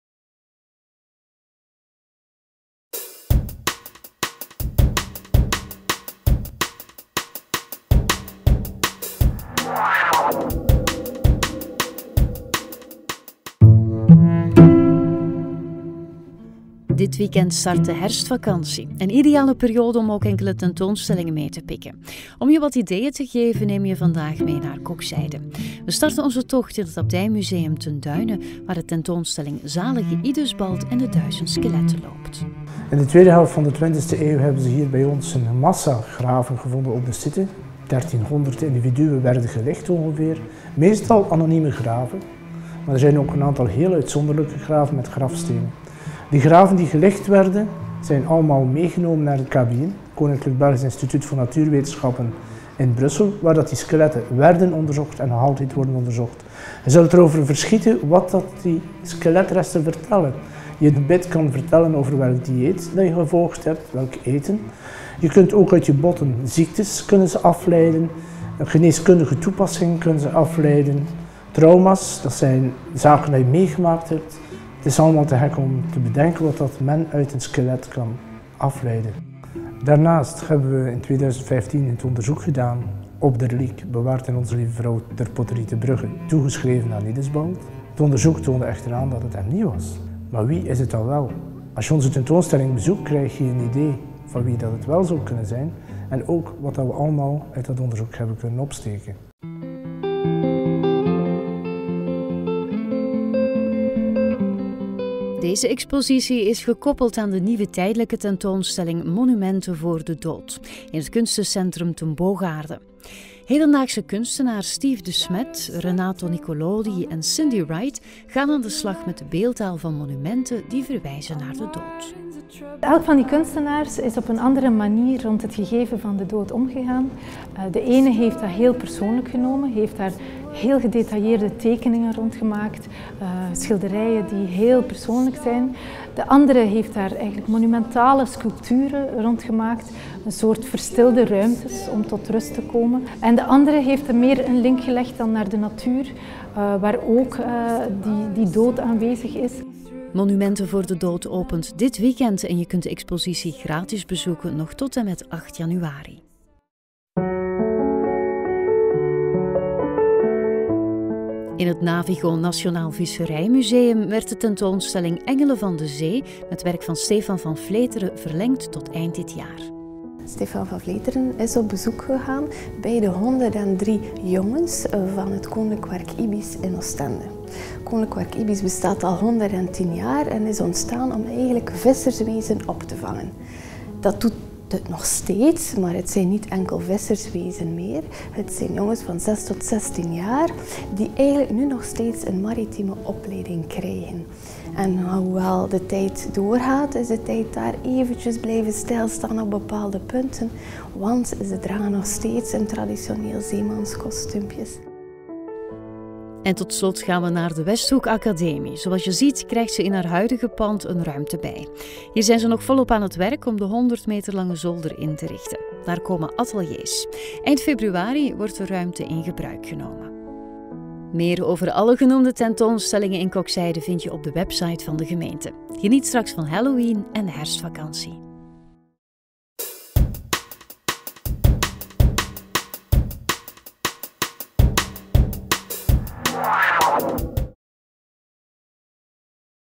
Trefpunt Koksijde oktober 2016 - tentoonstellingen
Trefpunt oktober zet de gemeentelijke expo’s in de kijker. Bijzonder, want in beide expo’s kan je hoofden zien. In het NAVIGO-Nationaal Visserijmuseum gaat het om de portretten van Stefaan Vanfleteren ‘Engelen van de Zee’. In het Abdijmuseum staan andere hoofden in de kijker. Zoals dat van abt Idesbald… of was het toch iemand anders? Ook de nieuwe expo van het Kunstencentrum Ten Bogaerde ‘Monumenten van de dood’ wordt in primeur voorgesteld!